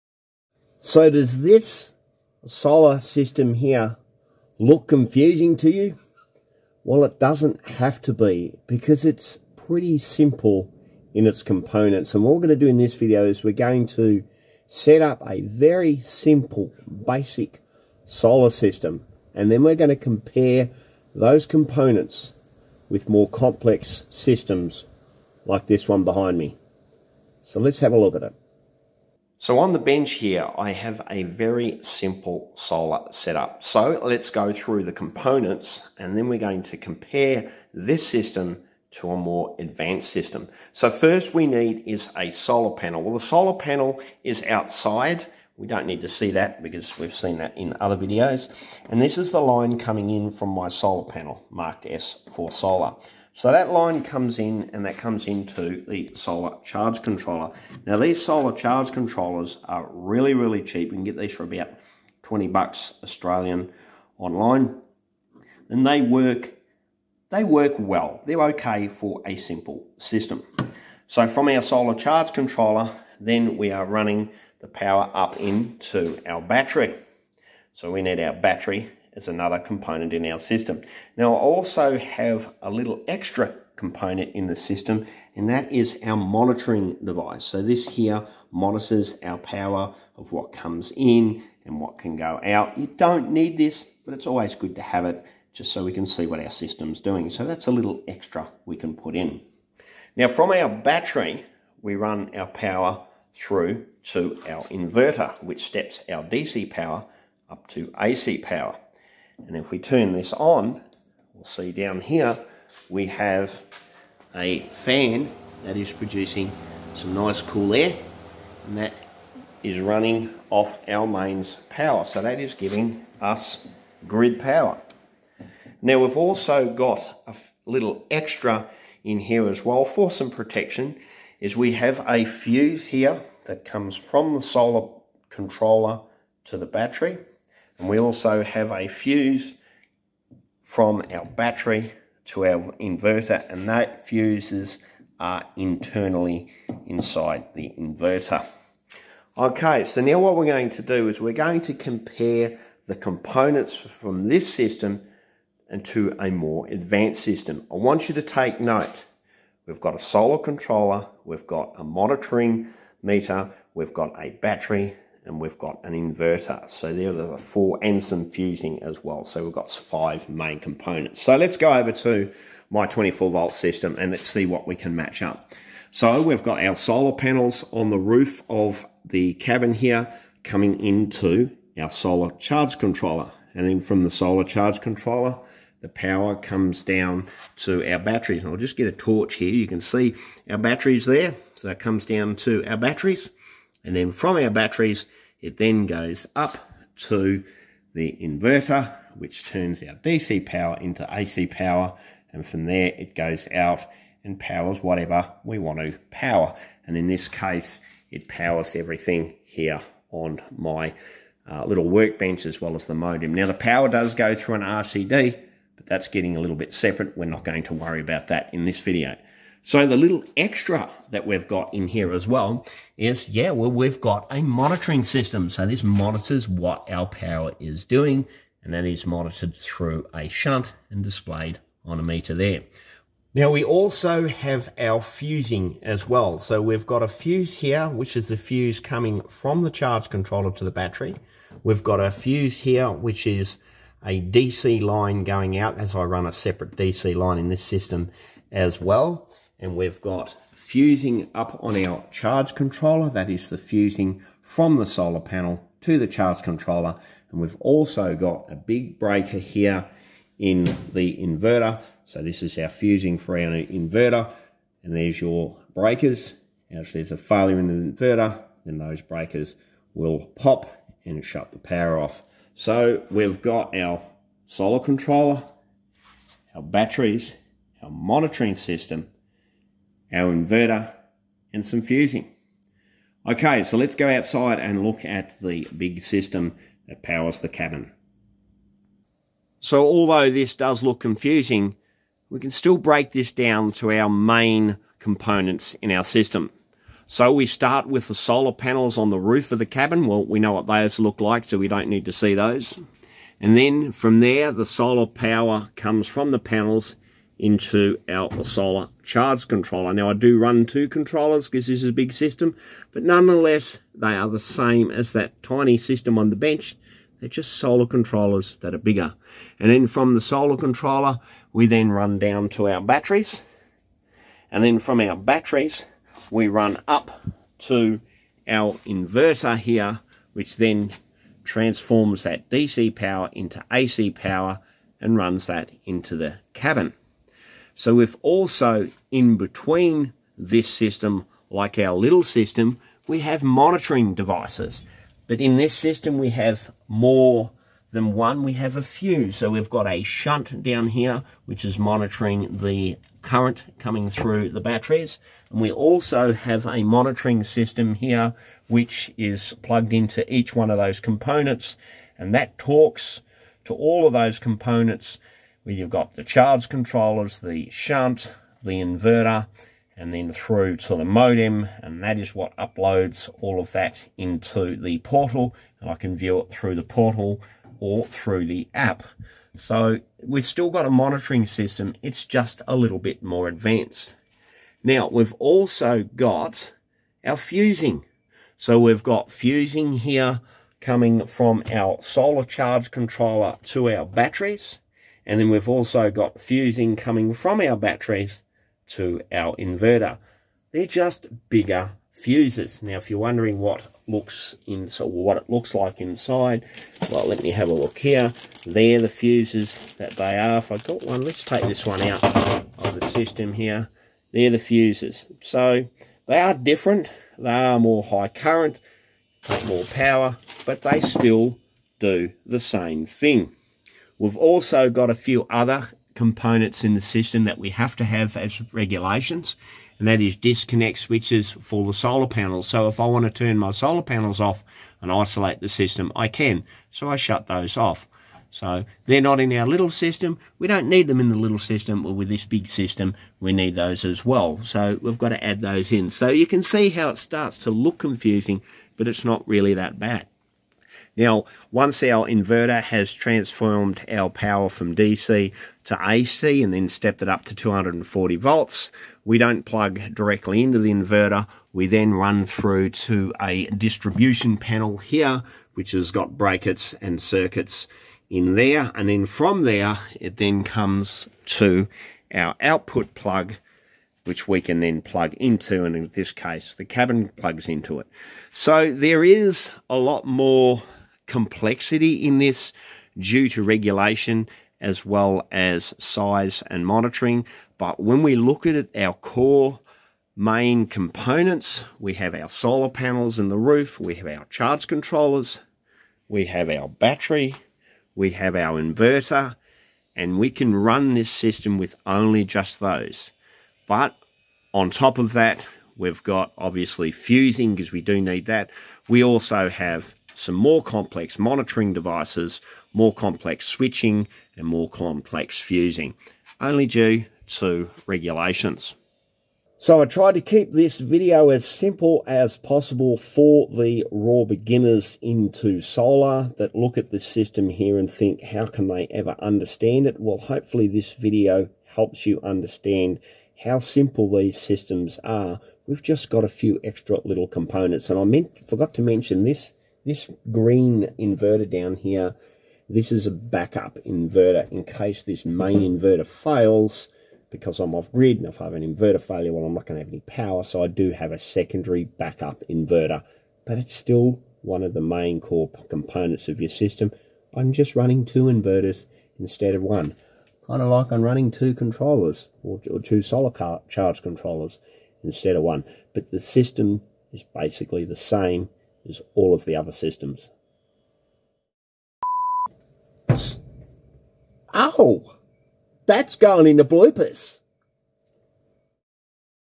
Off Grid Solar - Complex Vs Simple System & How They Are Similar
In this video we look at a complex off grid solar setup Vs a simple off grid setup and compare the difference.